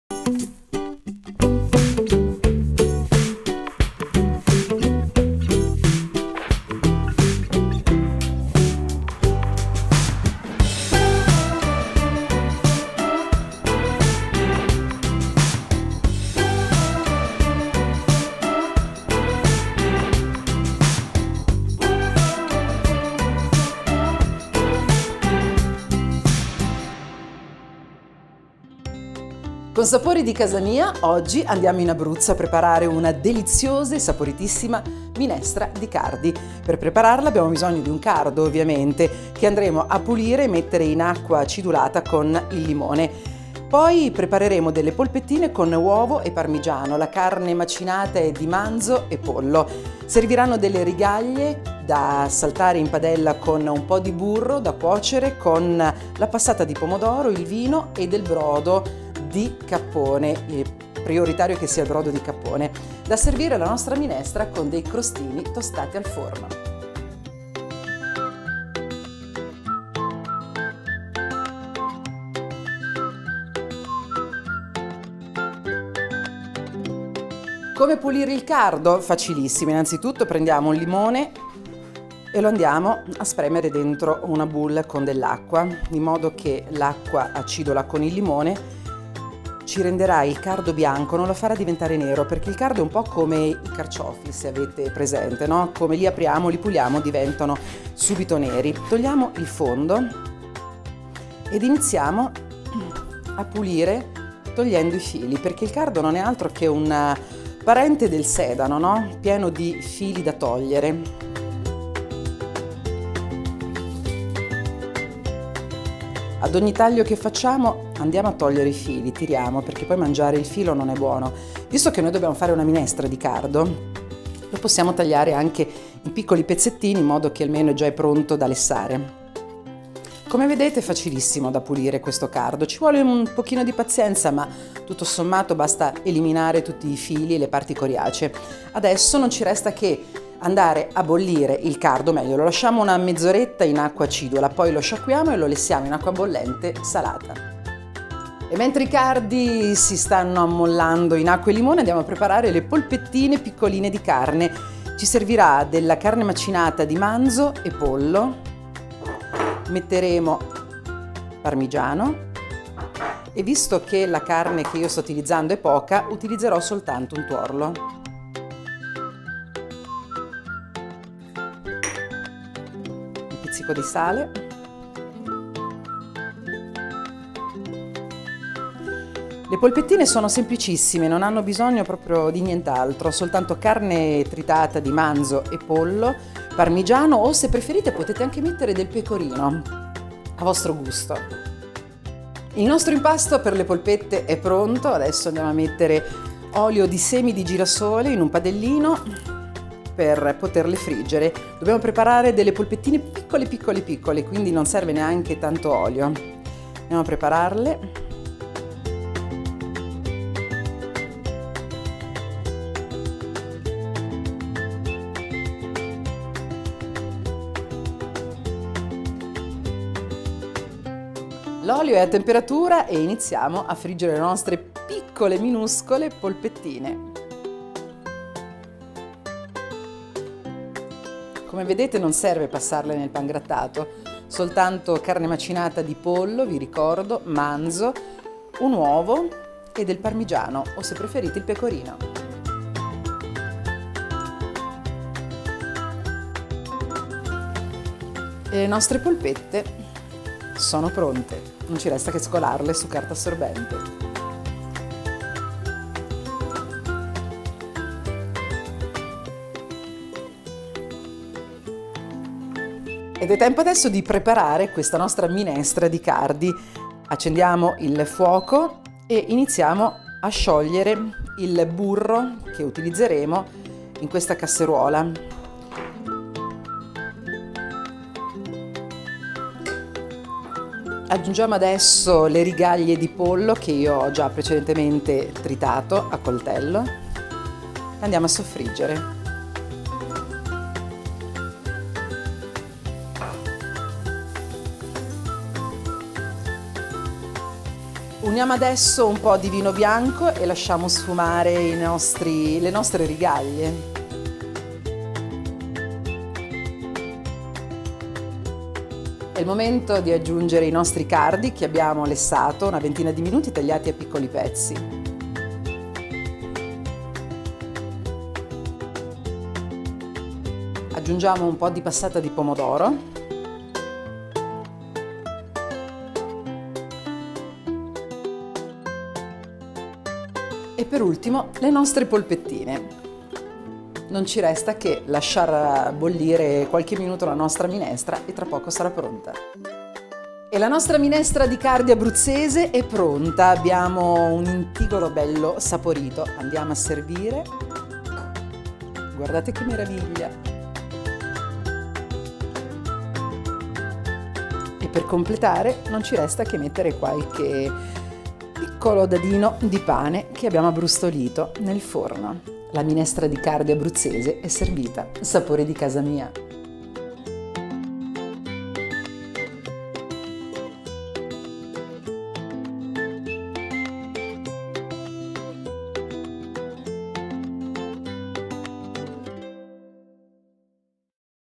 SaportiDiCasaMia: Minestra di Cardi
Oggi tappa in Abruzzo per una deliziosa e saporitissima minestra di cardi!

Con la nostra Daniela Rinaldi prepariamo uno dei piatti poveri legati alla tradizione contadina abbruzzese. Il protagonista di questa piatto è il cardo, un ortaggio invernale dal sapore simile a quello del carciofo, ma che nella forma e nel colore ricorda il sedano. Se non lo ha mai assaggiato e sei curioso/a di scoprire il suo gusto, questa è la ricetta giusta da provare!

Non vedi l'ora di gustarla? Scopri subito la ricetta. Buona visione!

TEMPO DI PREPARAZIONE: 3 ore

INGREDIENTI:
✅ Un cardo di grosse dimensioni
✅ 200 gr di carne macinata di manzo o pollo
✅ 120 gr di rigaglie di pollo o tacchino
✅ 2 litri di brodo di cappone o gallina
✅ 50 gr di formaggio grattugiato
✅ 2 uova
✅ 2 cucchiai di passata di pomodoro
✅ 1 dl di vino bianco secco
✅ Sale q.b.
✅ Pane raffermo tagliato a dadini e tostato in forno
✅ Una spolverata di parmigiano o pecorino grattugiato

PROCEDIMENTO

1. Pulite il cardo, tagliatelo a pezzi e mettetelo a bagno in acqua fredda e succo di limone per un paio d’ore, così perderà il gusto amarognolo; lessatelo e scolatelo al dente.

2. A parte impastate le uova con la carne macinata e il formaggio, formando delle piccolissime polpettine che friggerete in olio.

3. Pulite le rigaglie di pollo o tacchino, tagliatele a pezzi e all’interno di una casseruola dai bordi alti fatele rosolare nel burro, poi unite il vino bianco e fate sfumare. A questo punto aggiungete i cardi, la passata di pomodoro, il brodo e il sale, portando a ebollizione.

4. Servite la minestra con la dadolata di pane e con una spolverata di parmigiano o pecorino grattugiato.